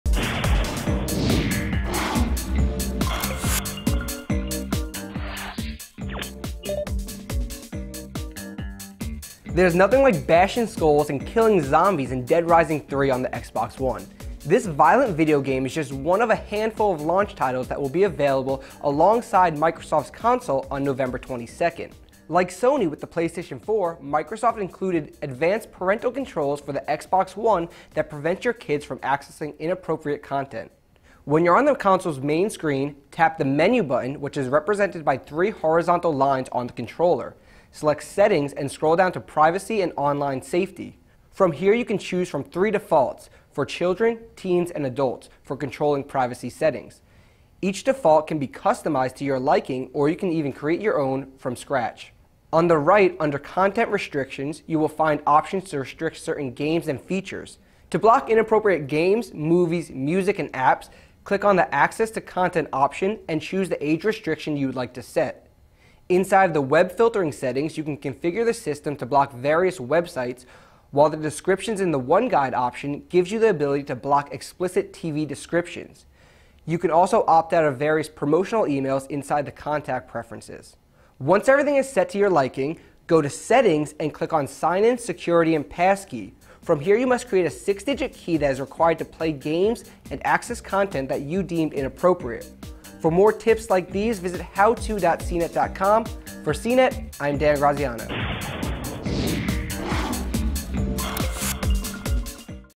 CNET How To - Set up parental controls on the Xbox One
Inappropriate games, movies, music, apps, and more can be blocked using Microsoft's advanced parental controls. Here's what you need to know.